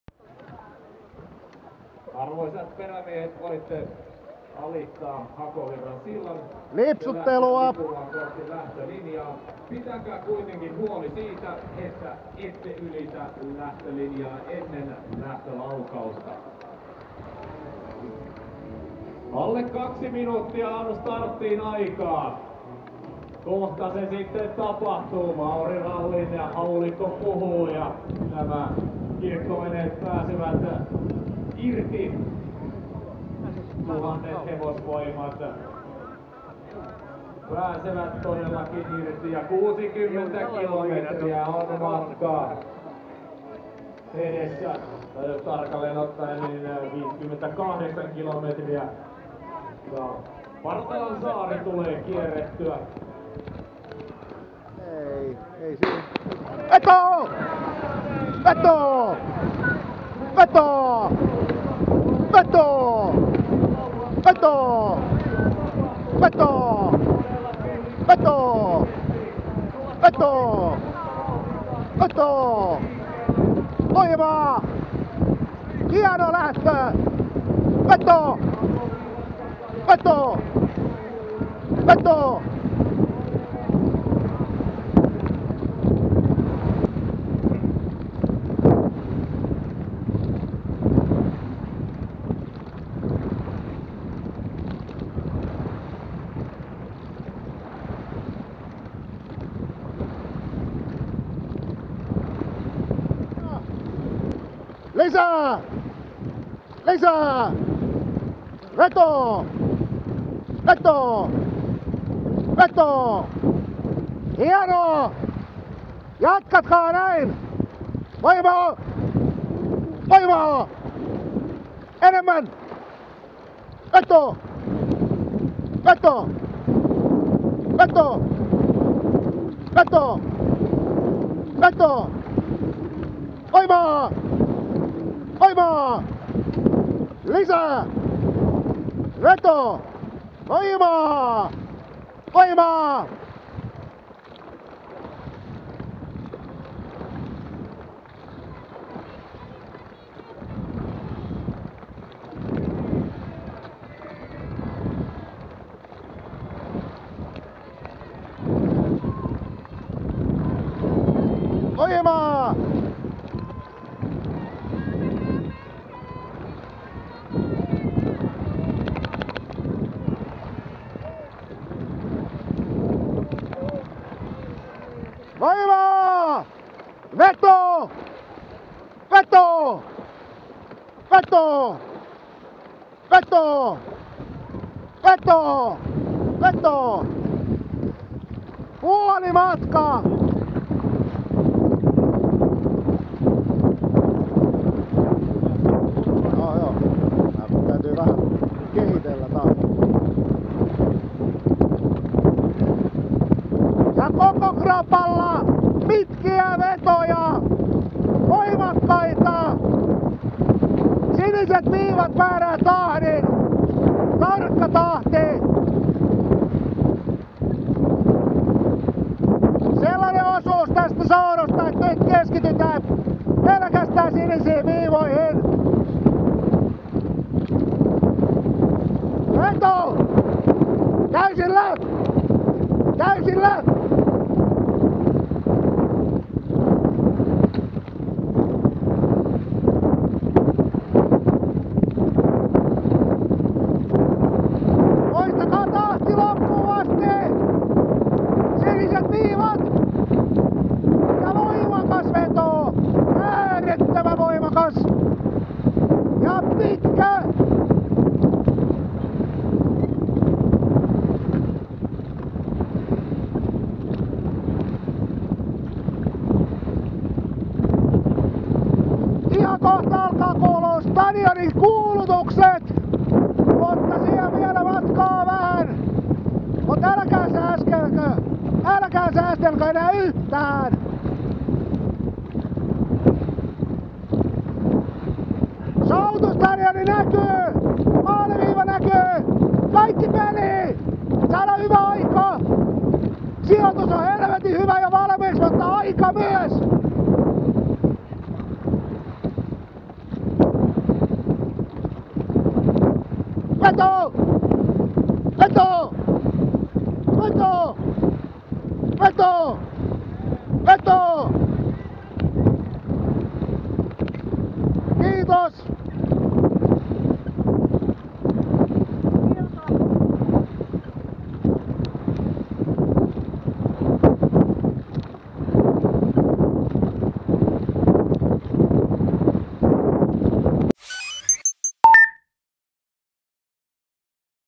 Copperiina Sulkavan suursouduissa 7.7.2018
Satakuntalaisen Copperiinan sekajoukkueen soutua Sulkavan suursouduissa 7.7.2018